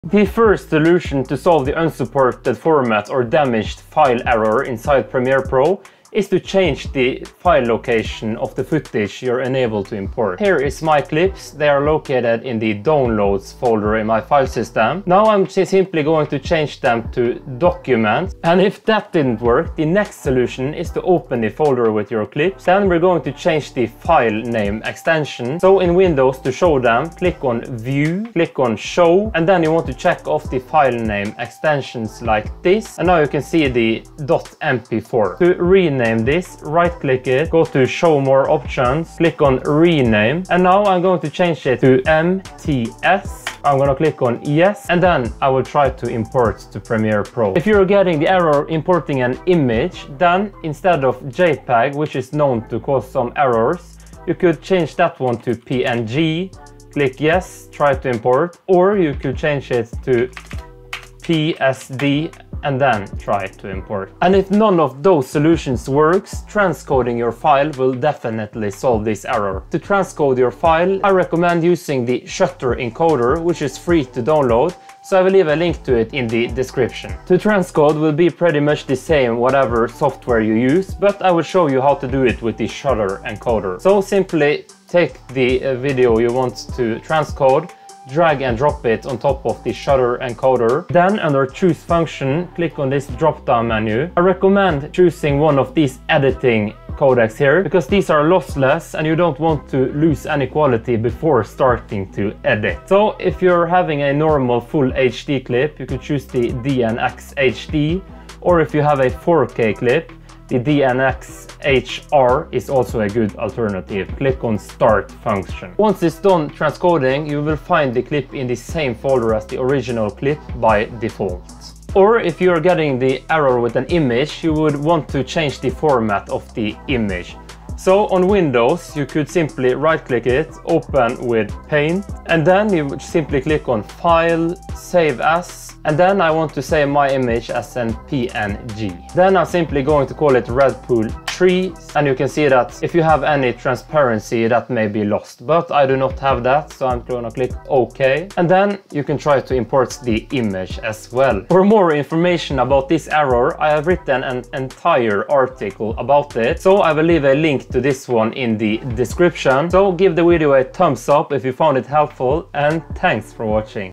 Fix “Unsupported Format or Damaged File” in Premiere Pro
The "Unsupported Format or Damaged File," is a common error message to see within the import errors in Premiere Pro. In this video you'll learn how to quickly fix it.